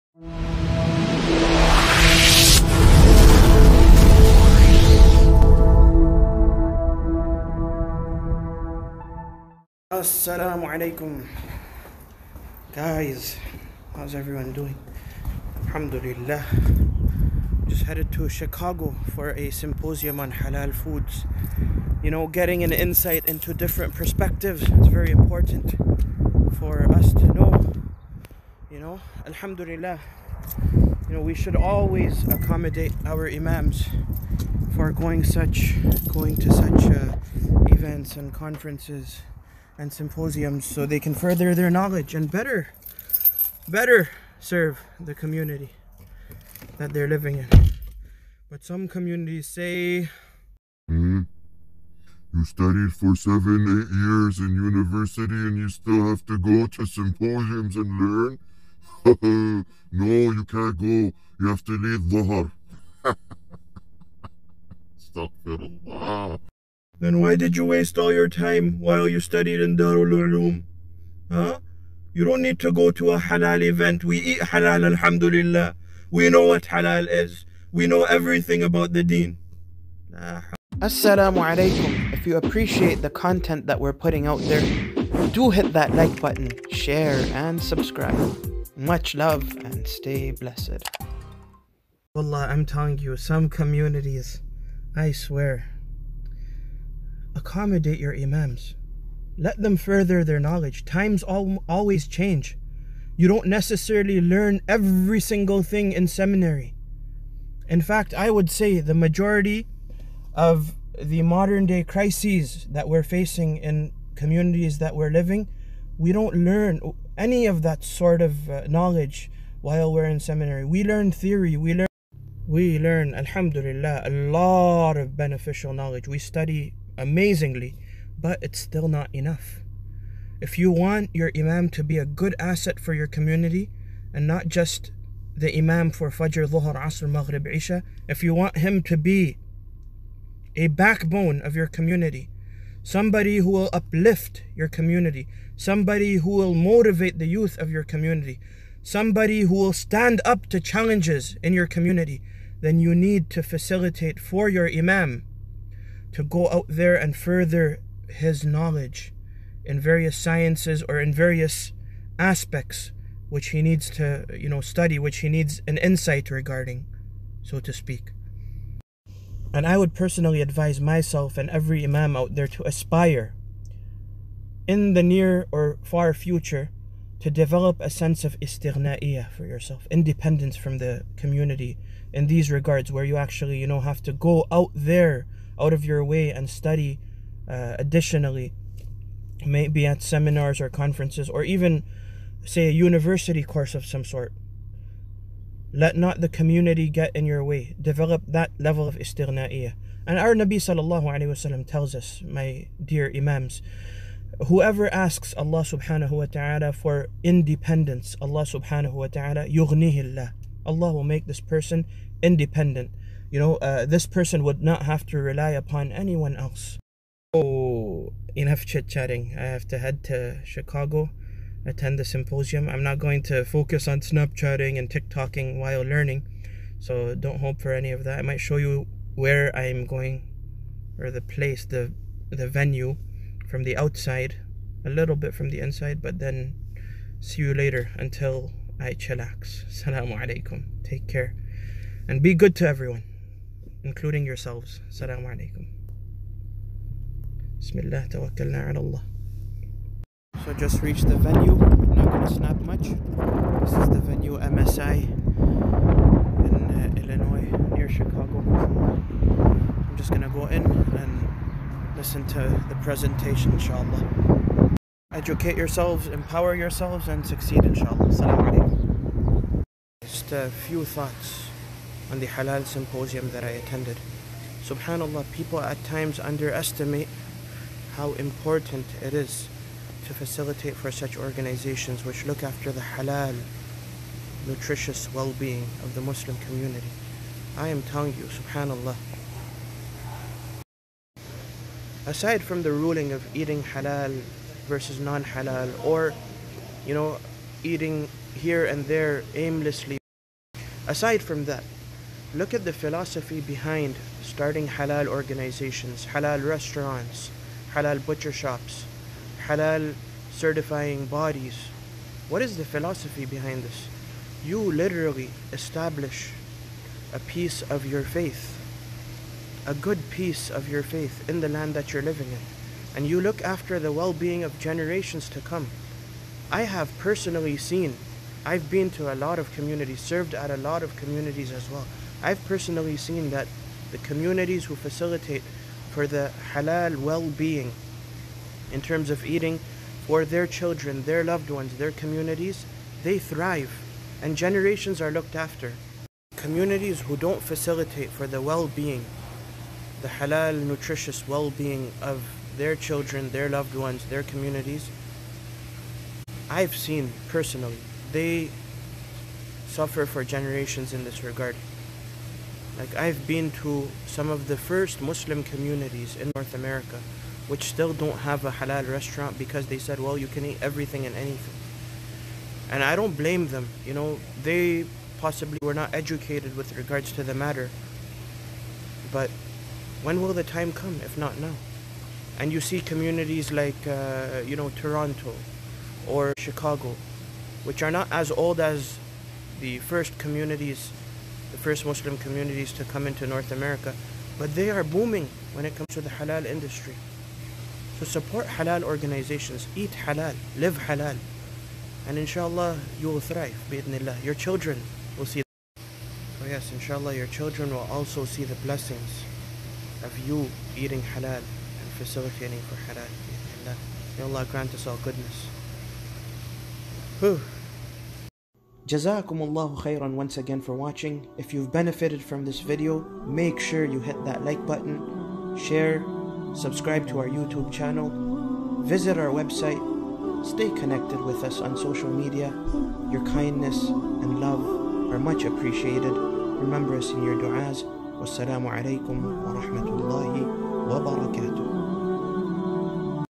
Halal Eating Preserves Generations | Imam Mazhar Mahmood
Recorded during a halal symposium, Imam Mazhar Mahmood speaks to the importance of establishing halal restaurants, eateries and organizations that facilitate for the dietary well-being of the Muslim society and future Muslim generations.

He also highlights his observations regarding halal vs non halal eating communities in relation to their dietary habits.

Imam Mazhar also mentions the importance of community leaders becoming independent at their respective capacities and finding avenues to further their knowledge and expertise. This will prove to be a great means of personal development, progress and it'll serve as a great benefit for the communities that they reside in.

Much love and stay blessed ✌🏻🤲🏻